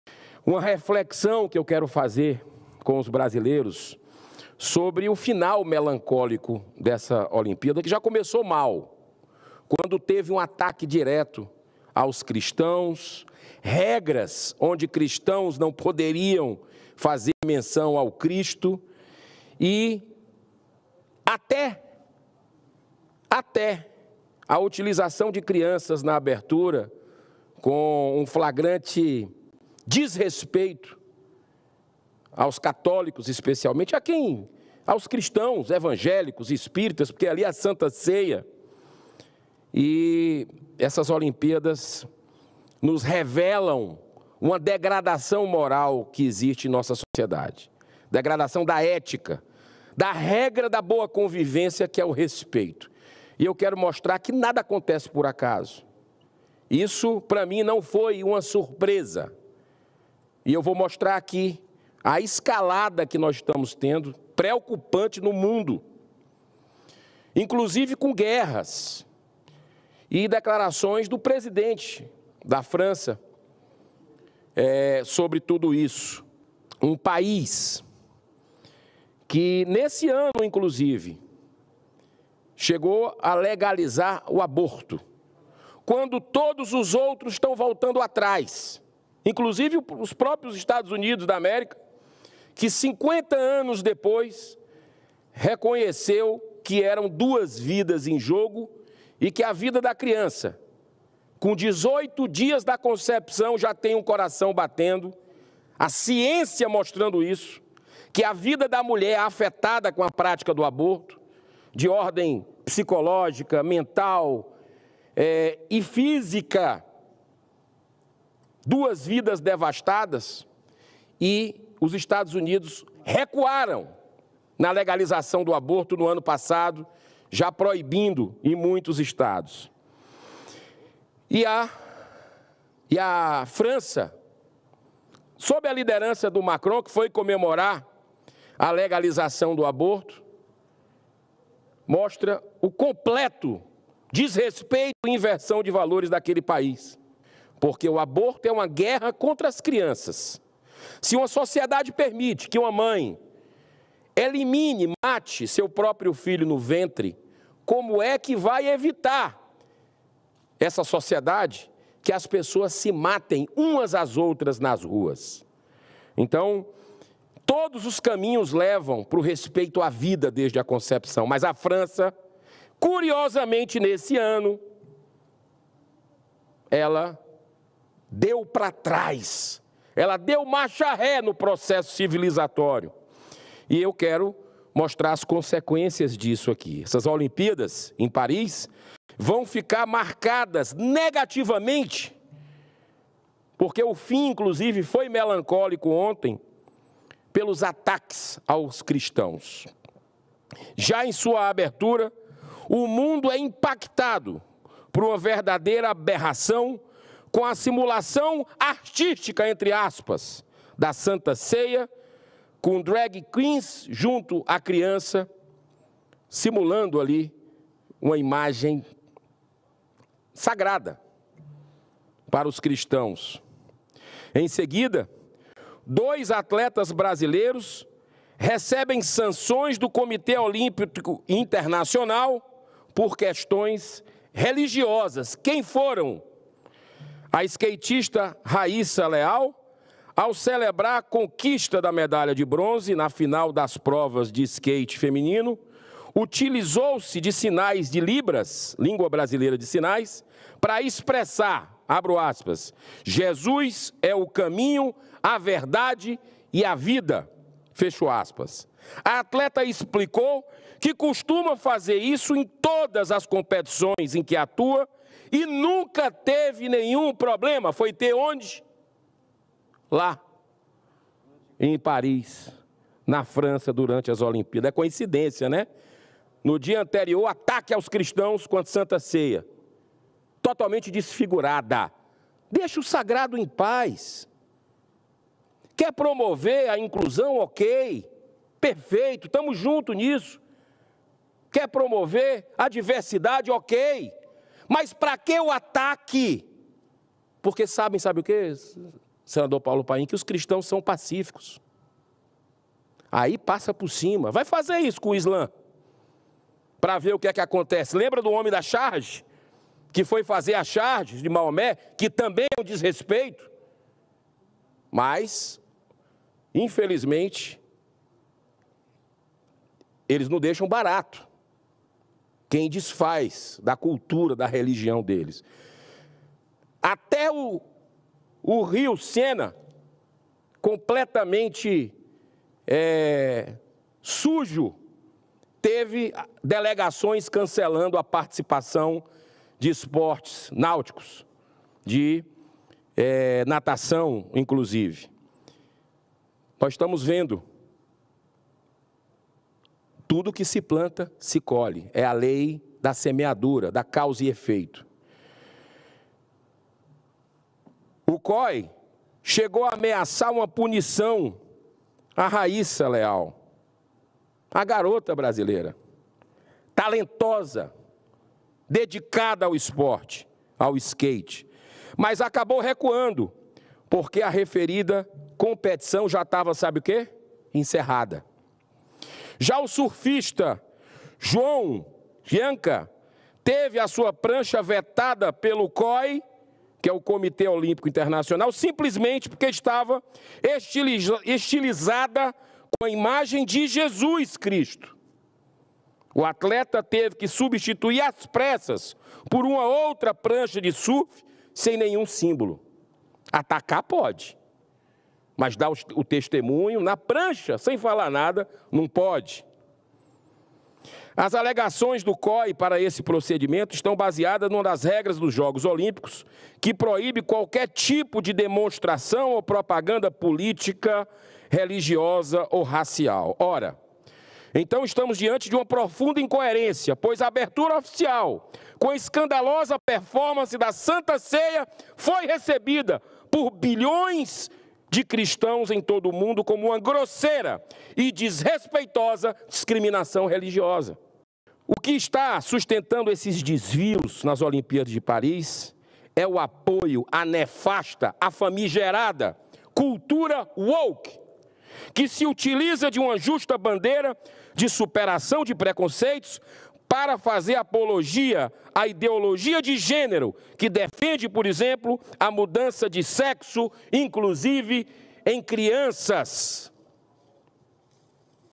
GIRÃO FAZ CRÍTICAS AOS JOGOS OLÍMPICOS DE PARIS: "DEGRADAÇÃO MORAL"
O senador Eduardo Girão (Novo-CE) criticou às Olimpíadas de 2024 na França, apontando intolerância religiosa contra cristãos, citando inclusive casos contra brasileiros, e exaltação da 'cultura woke', durante sua fala no plenário do Senado nesta segunda-feira (12). Girão ainda criticou o país e seu presidente, Emmanuel Macron, por aprovar recentemente o aborto na Constituição.